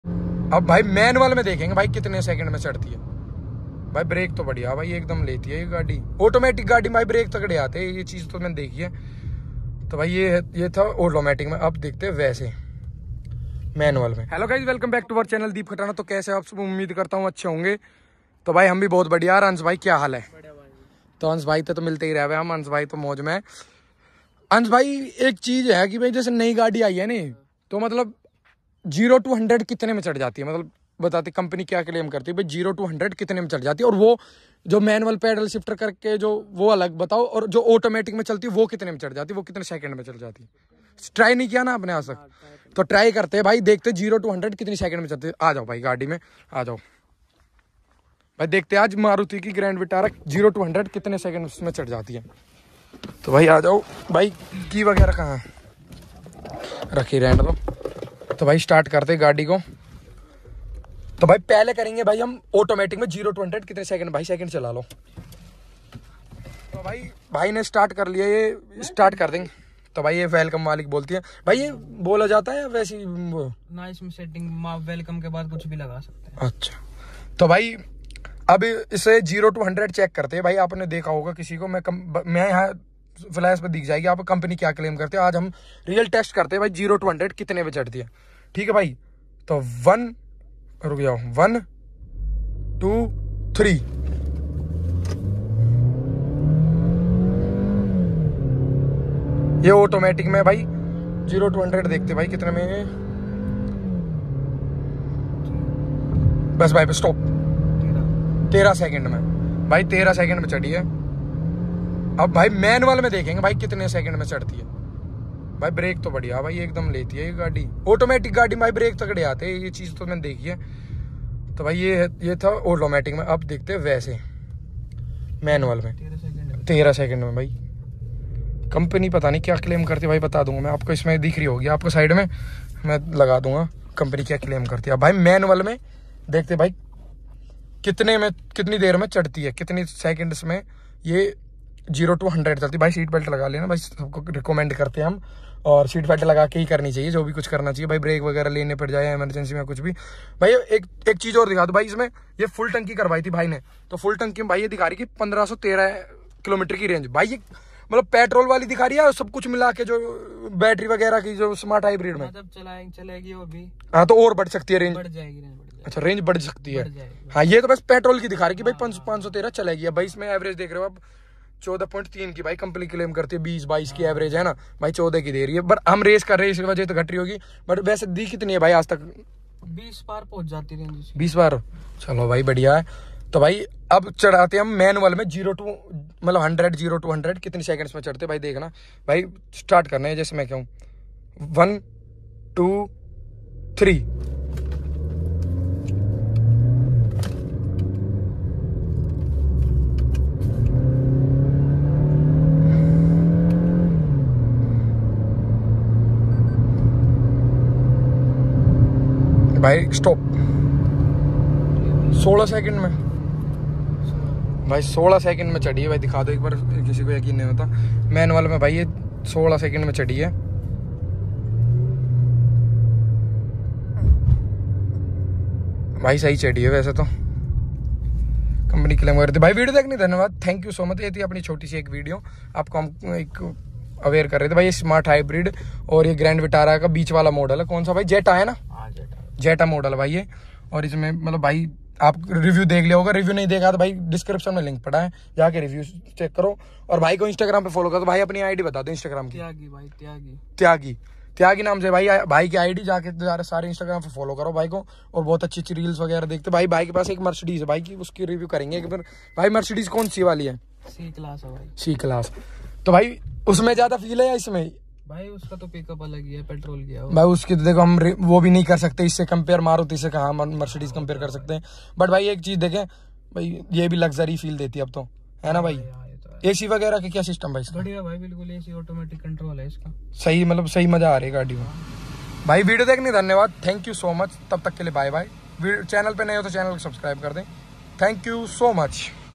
Grand vitara 0 to 100 check || problems in grand vitara ?? Deep kahtanaa
Grand vitara 0 to 100 check || problems in grand vitara ?? Deep kahtanaa @ArunPanwarx @arunpanwarindri @akashpanwar

Hope you enjoyed this videos.

Checking new Maruti Suzuki grand vitara break system in automatic smart hybrid with Ansh tyagi owner of the car.